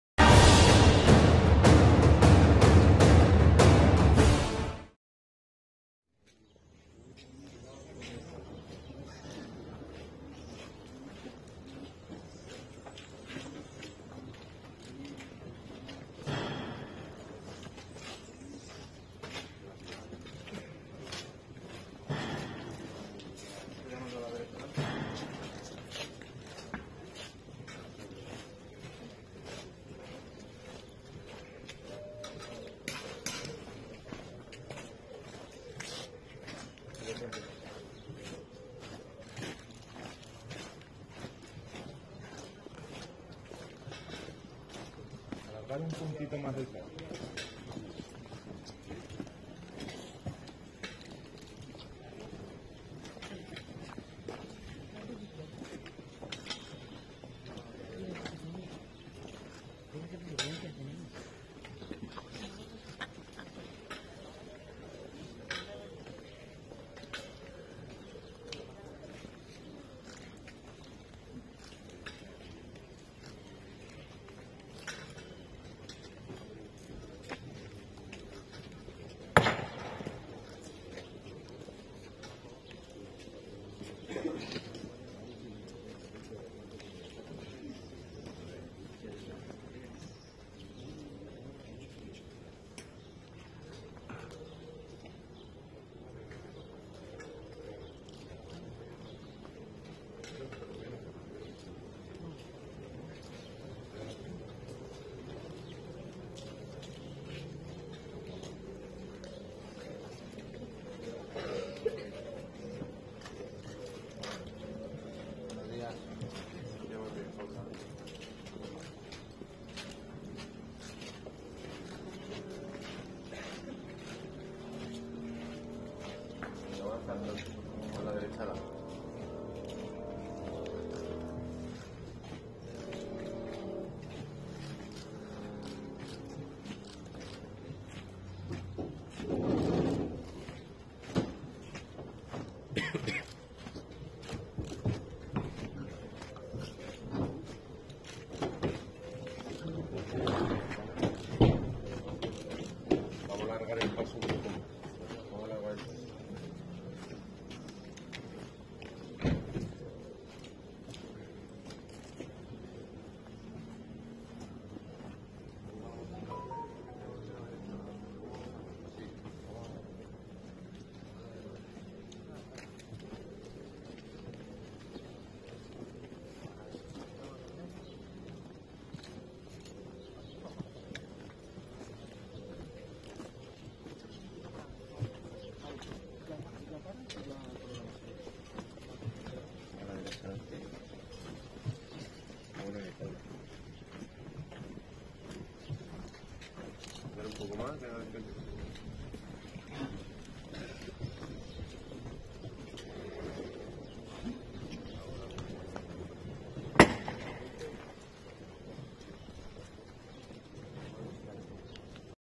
Salida traslado Jesús Despojado a la Purísima 2024, salamanca
Salida traslado Jesús Despojado y Caridad y Consuelo la mañana del domingo 17 de marzo de 2024 a la Iglesia de la Purísima para preparar la salida de un nuevo Domingo de Ramos.